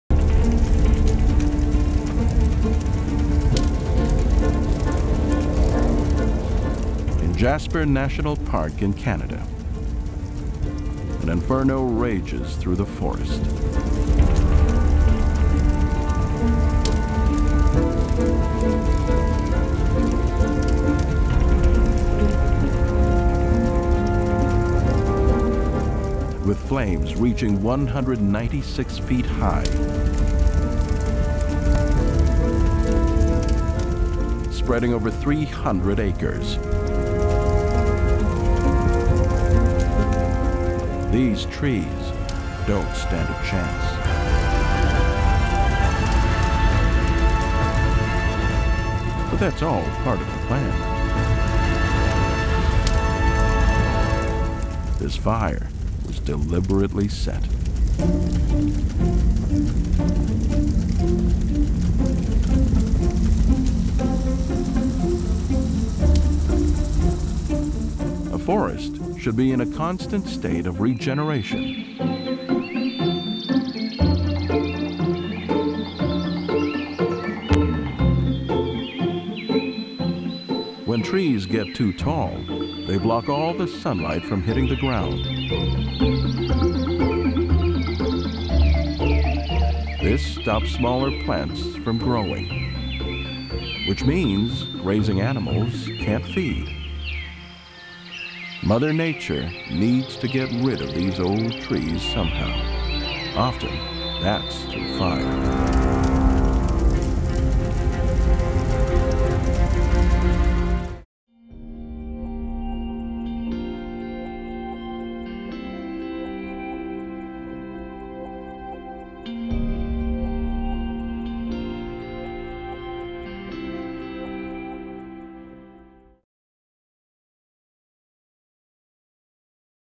Why This Raging Forest Fire is Nothing to be Alarmed About
Not all forest fires are a national emergency – this one, in Jasper National Park, was ignited on purpose to clear excess vegetation and create a healthier ecosystem.